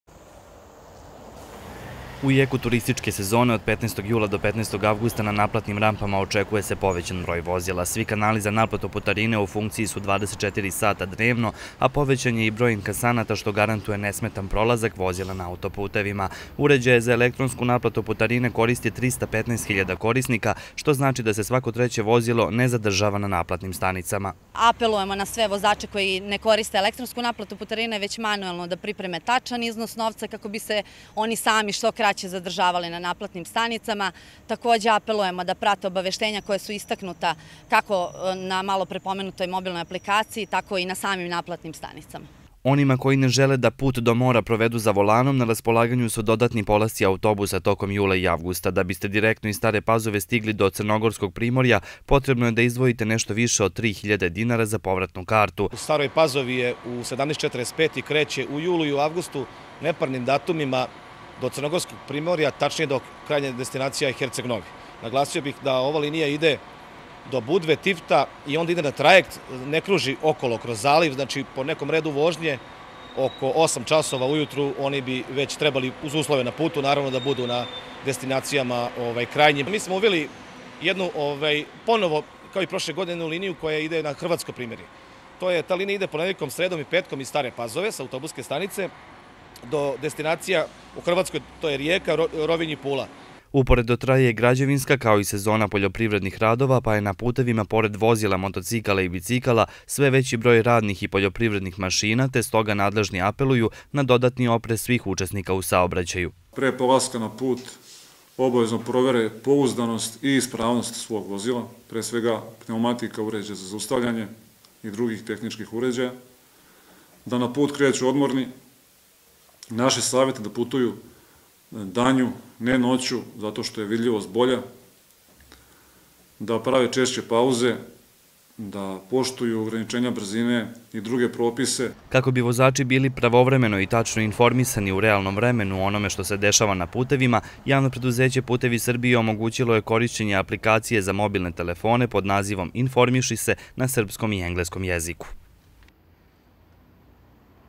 Počela sezona godišnjih odmora
Sezona godišnjih odmora uveliko je počela zbog čega je režim saobraćaja pojačan pre svega na auto-putevima, kao i putevima koji vode ka Makedoniji, Crnoj Gori i Bugarskoj. S obzirom da je u toku letnja turistička, zatim građevinska i sezona poljoprivrednih radova, na putevima je sve više vozila, ali i biciklisa, motocklista i poljoprivrednih mašina.